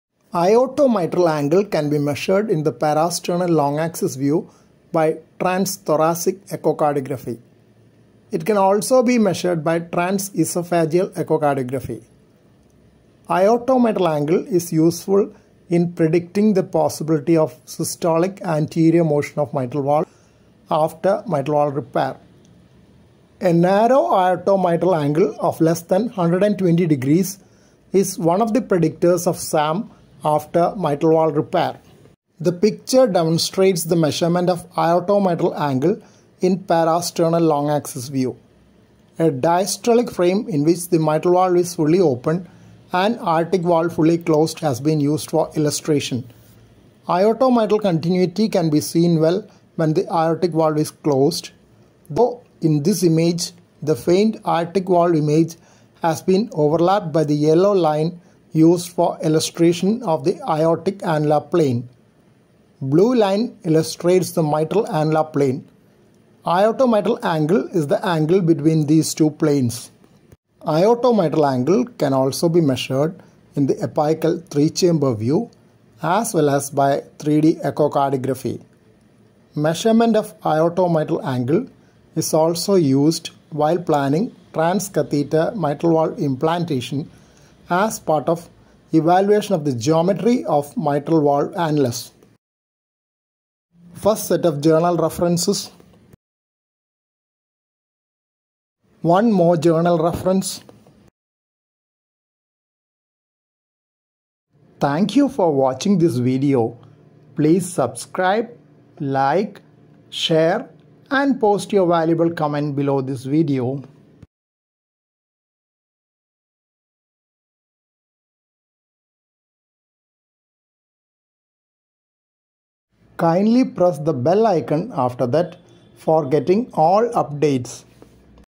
Measurement of aorto mitral angle by echo
Aorto-mitral angle can be measured in the parasternal long axis view by trans thoracic echocardiography. It can also be measured by trans esophageal echocardiography. Aorto-mitral angle is useful in predicting the possibility of systolic anterior motion of mitral valve after mitral valve repair. A narrow aorto-mitral angle of less than 120 degrees is one of the predictors of SAM after mitral valve repair. Aorto-mitral angle is also measured as part of evaluation of mitral annulus prior to TMVR.